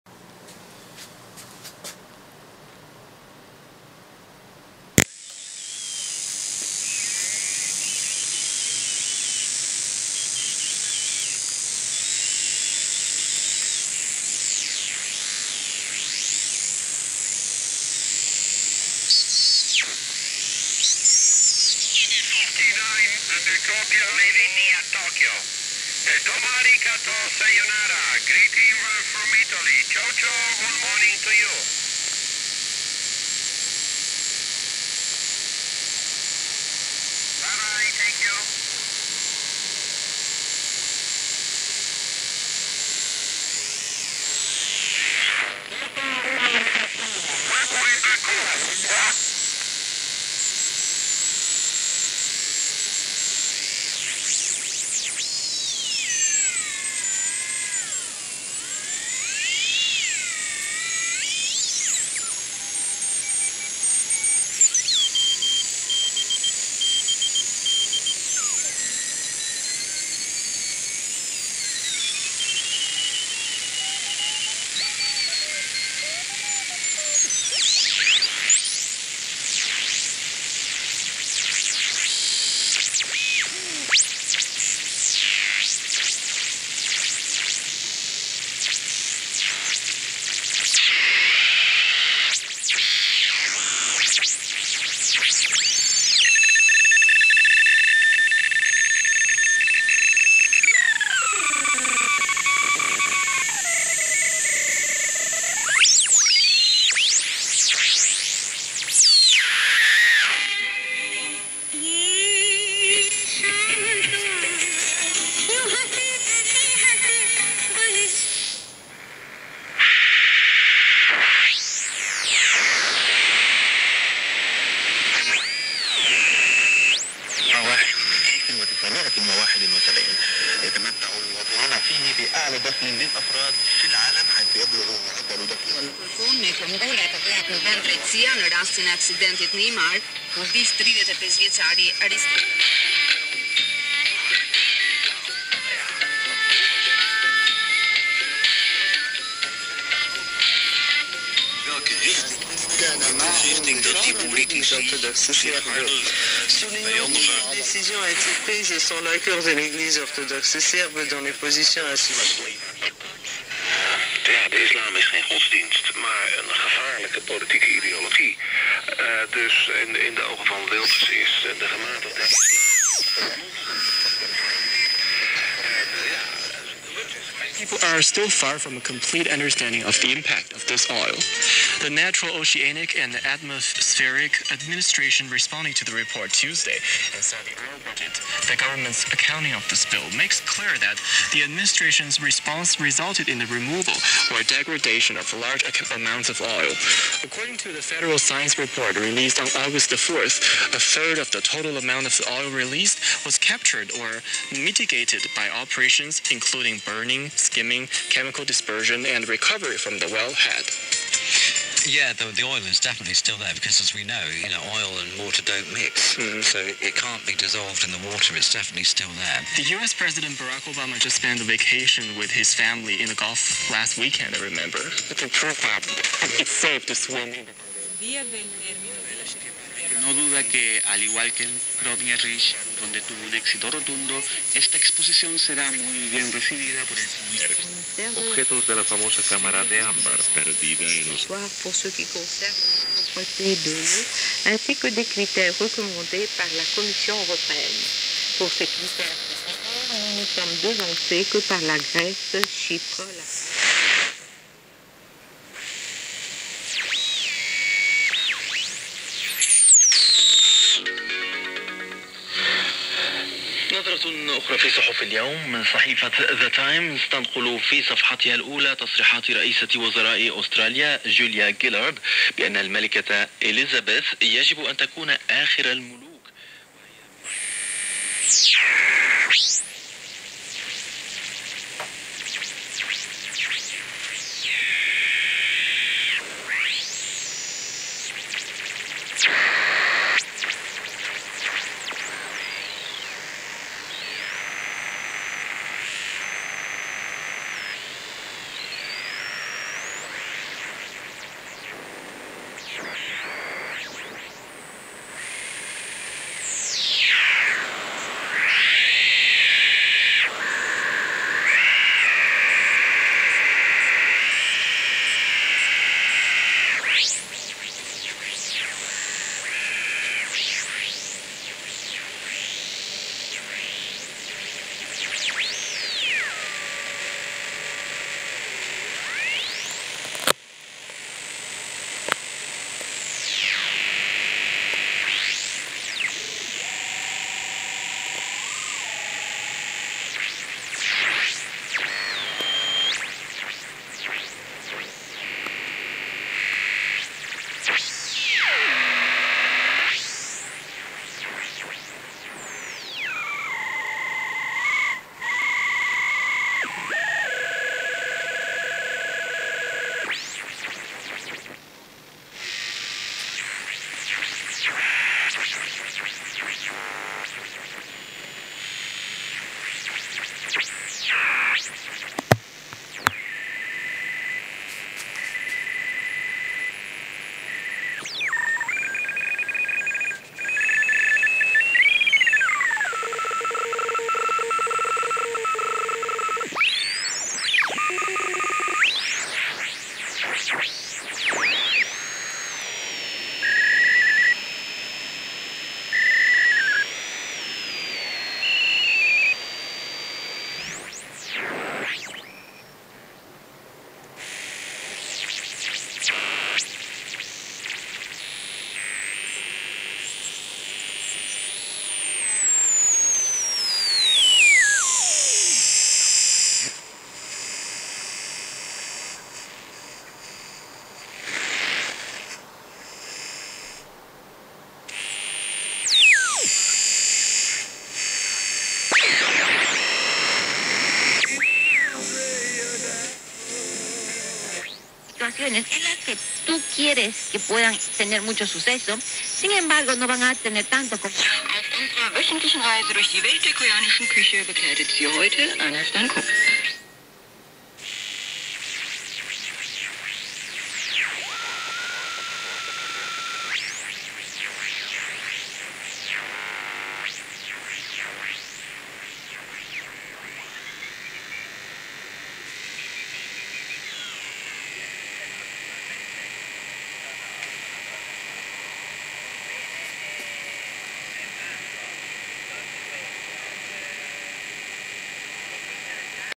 Twinplex Regen Receiver.avi
Shortwave regen receiver using a Twinplex circuit with 19 tube.
With this coil about 2 - 7 Mhz frequency coverage.
5 meters wire antenna.